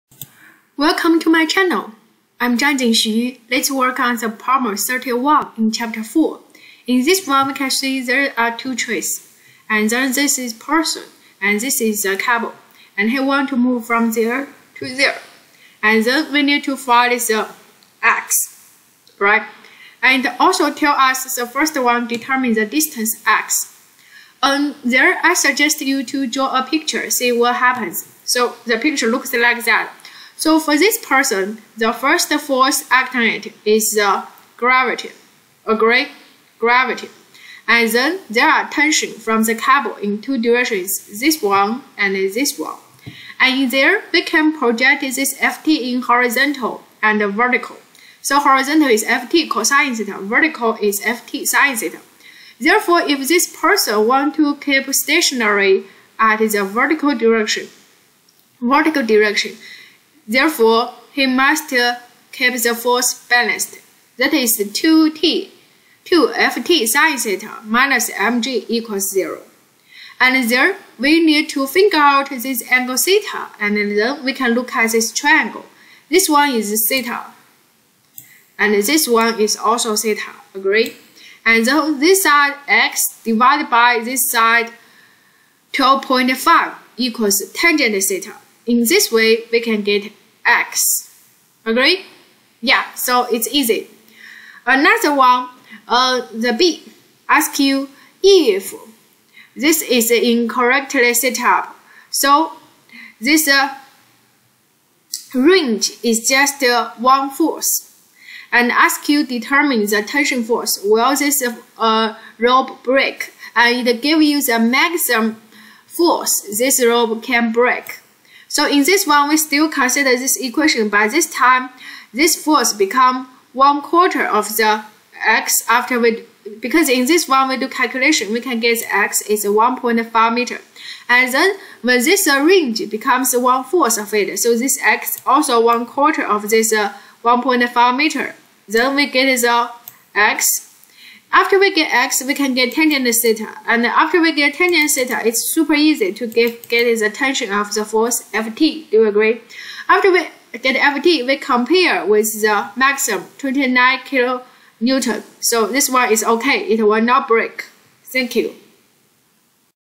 (4-31) Christian is making a Tyrolean traverse as shown in Fig. 4-35. That is, he traverses a chasm
(4-31) Christian is making a Tyrolean traverse as shown in Fig. 4-35. That is, he traverses a chasm by stringing a rope between a tree on one side of the chasm and a tree on the opposite side, 25 m away. The rope must sag sufficiently so it won’t break. Assume the rope can provide a tension force of up to 29 kN before breaking, and use a “safety factor” of 10 (that is, the rope should only be required to undergo a tension force of 2.9 kN) at the center of the Tyrolean traverse, (a) Determine the distance x that the rope must sag if it is to be within its recommended safety range and Christian’s mass is 72.0 kg. (b) If the Tyrolean traverse is incorrectly set up so that the rope sags by only one-fourth the distance found in (a), determine the tension force in the rope. Will the rope break?